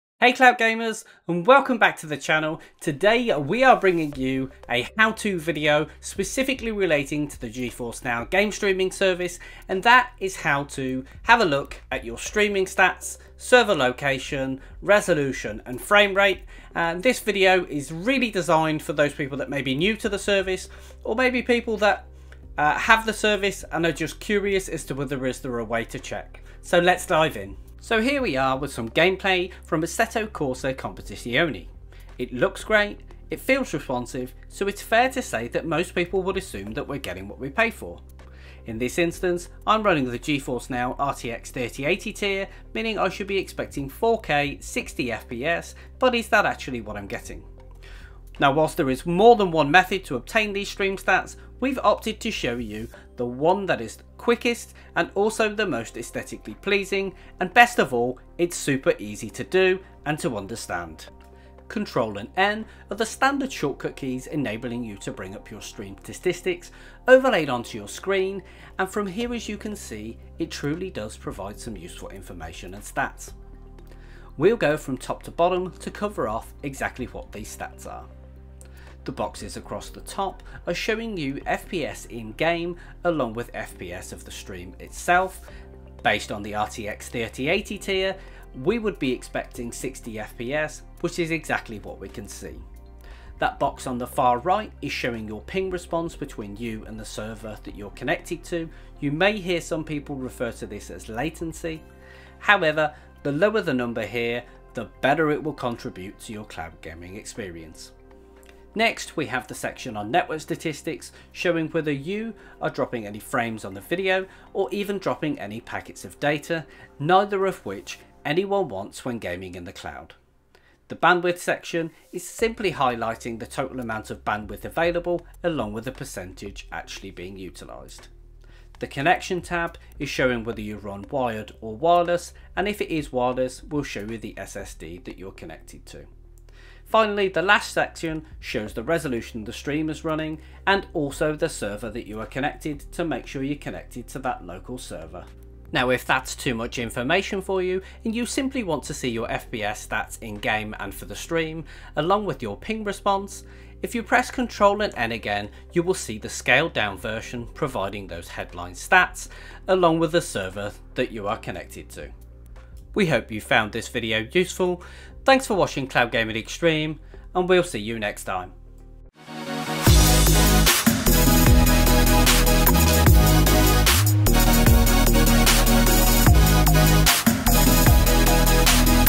How to Easily View Stream Stats on GeForce NOW
We show you how to view your stream stats in a quick and easy way that looks pretty aesthetically pleasing.

Using EPIC Games? Our Creator Code is CGXTREME

Amazon Luna